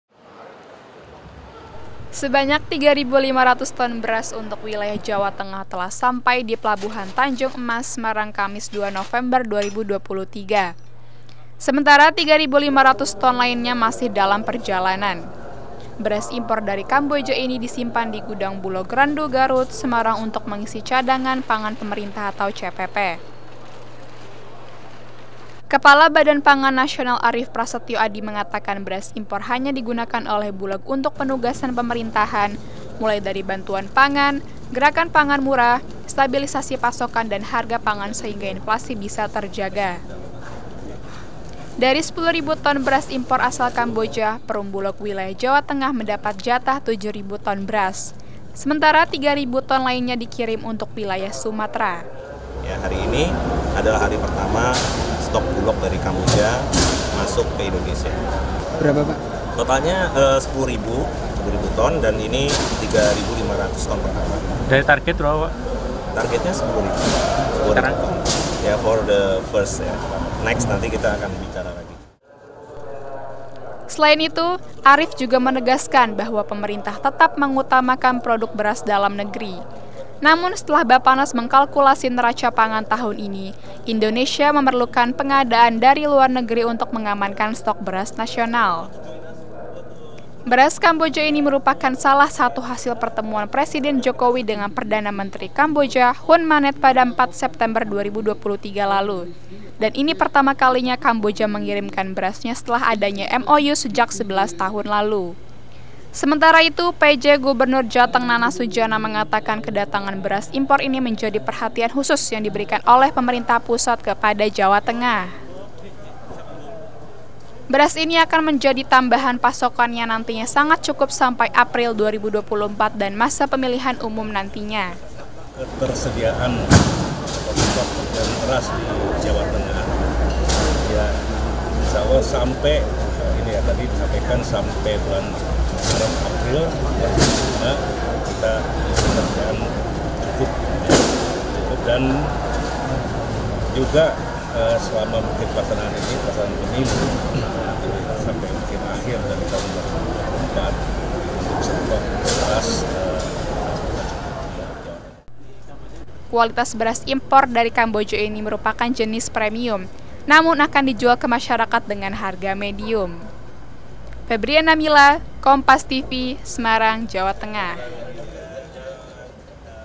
Ribuan Ton Beras Impor dari Kamboja Tiba di Jateng
SEMARANG, KOMPAS.TV - Sebanyak 3.500 ton beras untuk wilayah Jawa Tengah telah sampai di Pelabuhan Tanjung Emas Semarang Kamis (2/11/2023), sementara 3.500 ton lainnya masih dalam perjalanan. Beras impor dari Kamboja ini disimpan di gudang Bulog Randugarut Semarang untuk mengisi cadangan pangan pemerintah (CPP).

Kepala Badan Pangan Nasional, Arief Prasetyo Adi, mengatakan, beras impor hanya digunakan oleh Bulog untuk penugasan pemerintahan, mulai dari bantuan pangan, gerakan pangan murah, stabilisasi pasokan dan harga pangan sehingga inflasi bisa terjaga.

Dari 10.000 ton beras impor asal Kamboja, Perum Bulog wilayah Jawa Tengah mendapat jatah 7.000 ton beras, Sementara itu, 3.000 ton lainnya dikirim untuk wilayah Sumatera.

"Hari ini, adalah hari pertama stok Bulog dari Kamboja masuk ke Indonesia dengan total 10.000 ton, dan ini 3.500 ton pertama," ujar Arief.

Selain itu, Arief juga menegaskan bahwa pemerintah tetap mengutamakan produk beras dalam negeri, namun setelah bapanas mengkalkulasi neraca pangan tahun ini, Indonesia memerlukan pengadaan dari luar negeri untuk mengamankan stok beras nasional. Beras Kamboja ini merupakan salah satu hasil pertemuan Presiden Jokowi dengan Perdana Menteri (PM) Kamboja Hun Manet pada 4 September 2023 lalu dan ini pertama kalinya kamboja mengirimkan berasnya setelah adanya MOU sejak 11 tahun lalu.

Sementara itu Pj Gubernur Jateng, Nana Sudjana mengatakan, kedatangan beras impor ini menjadi perhatian khusus yang diberikan oleh pemerintah pusat kepada Jawa Tengah. Beras ini akan menjadi tambahan pasokan yang nantinya sangat cukup sampai April 2024 dan masa pemilihan umum nantinya.

"Ketersediaan stok beras di Jawa Tengah insya Allah sampai bulan Maret-April 2025 cukup. Selama pelaksanaan pemilu dan akhir 2024 stok beras cukup di Jawa Tengah," jelas Nana Sudjana.

Kualitas beras impor dari Kamboja ini merupakan jenis premium namun akan dijual ke masyarakat dengan harga medium.